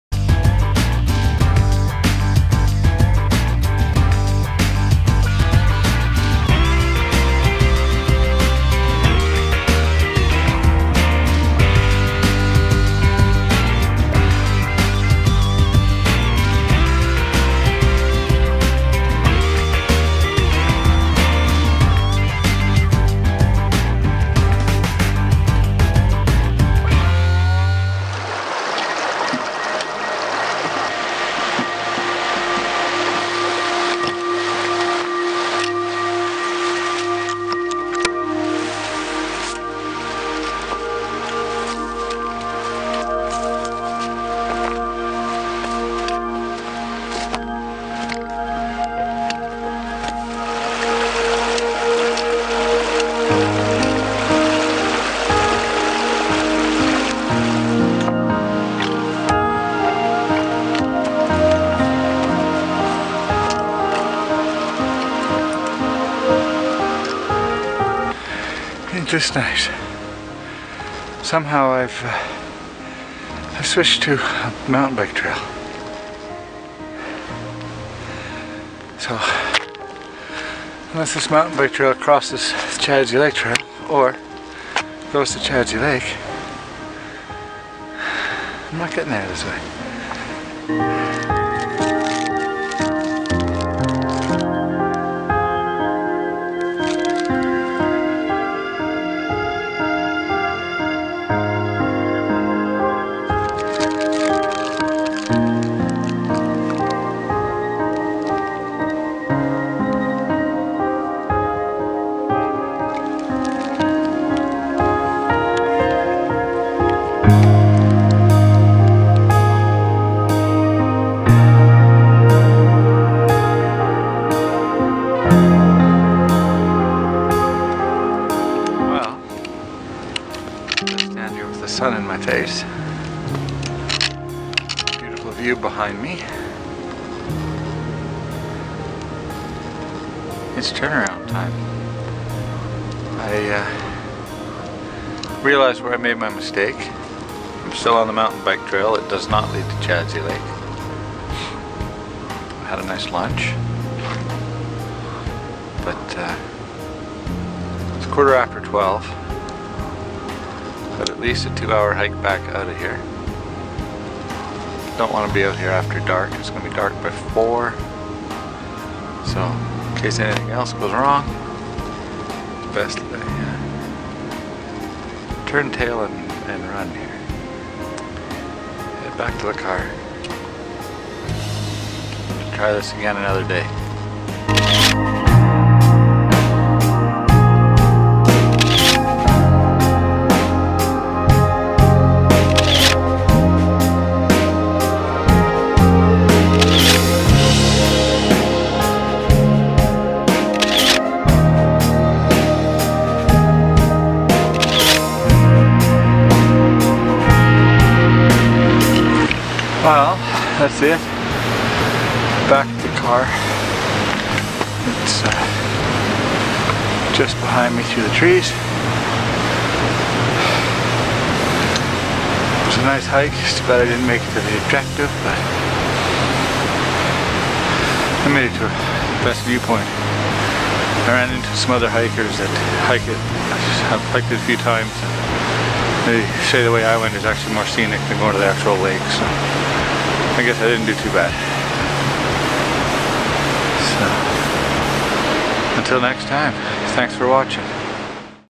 Sumas Mountain Day Hike
This is video from my new years day hike on Sumas mountain. Wrong turns and epic views made for a great start to 2015.